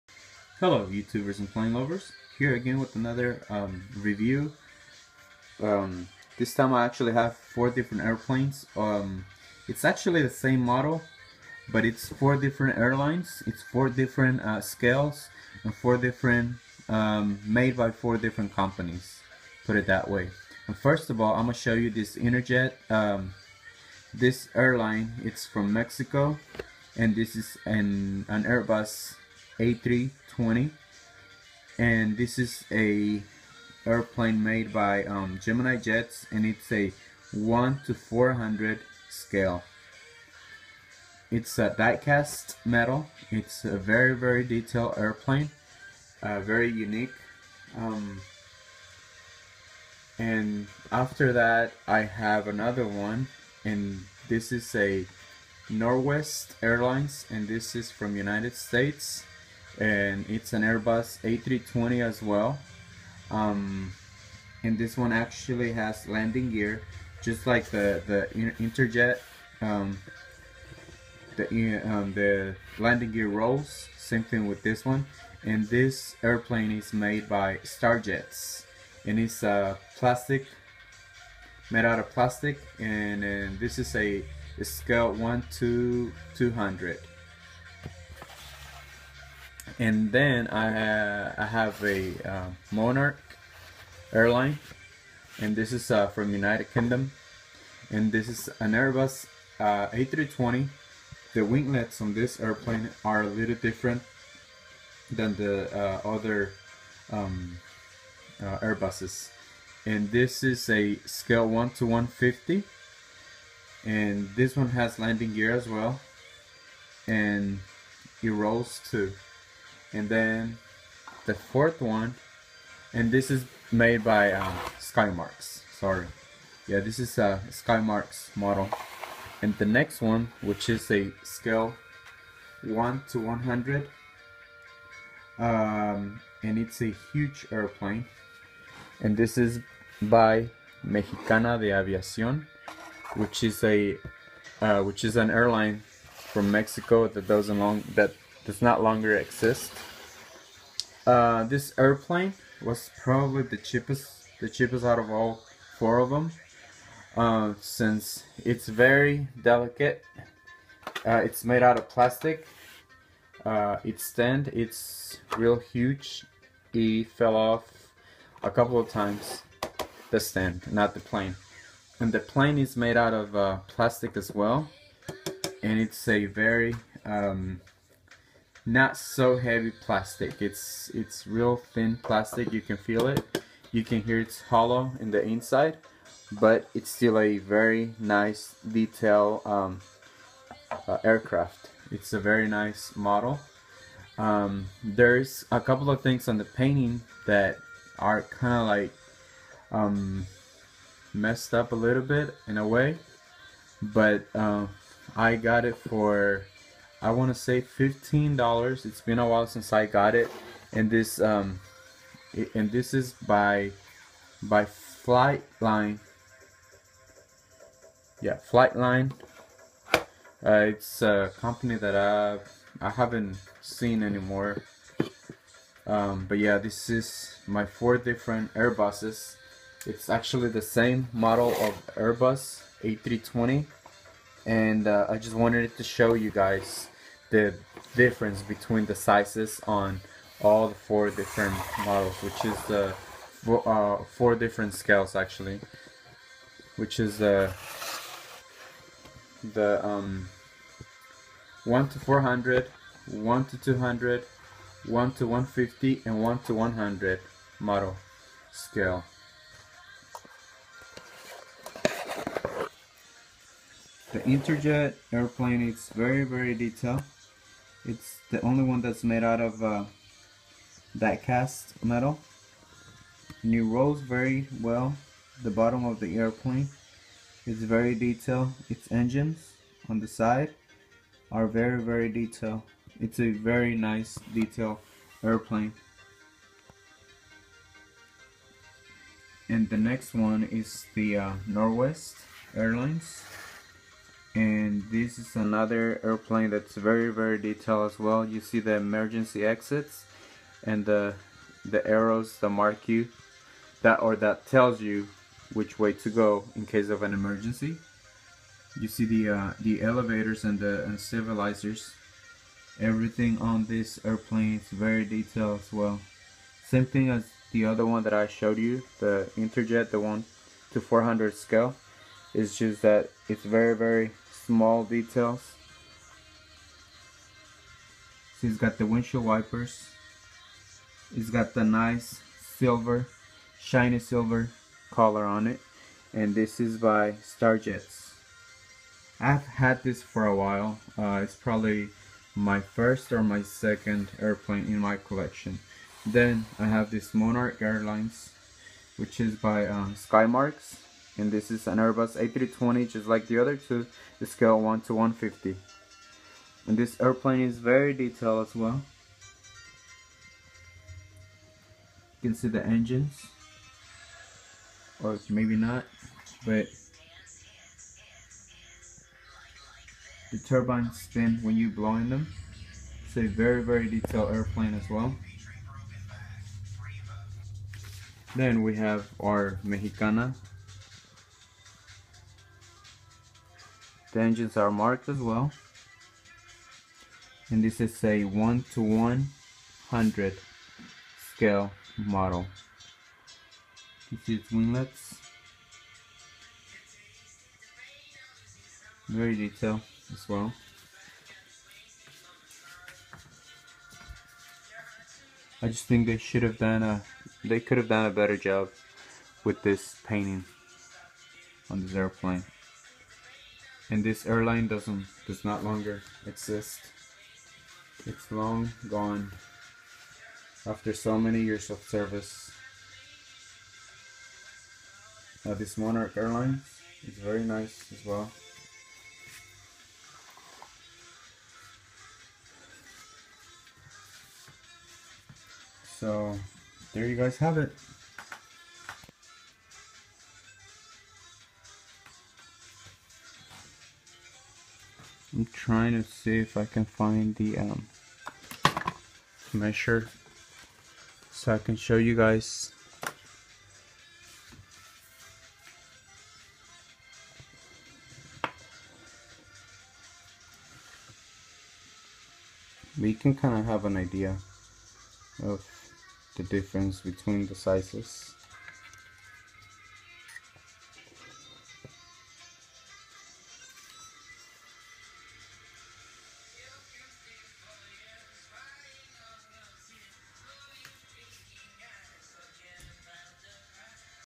Airbus A320 different scales
This is a video that will show you the difference between four Airbus A320 of scales 1:400 (4 x 3.5" / 10 x 9 cm), 1:200 (7.5 x 6.5" / 19 x 17 cm,) 1:150 (10 x 9.5" / 25.5 x 24 cm), 1:100 (15 x 13.5" / 38 x 34 cm). (Nose-Tail x wing spam). I hope this video helps you get an idea of the different sizes.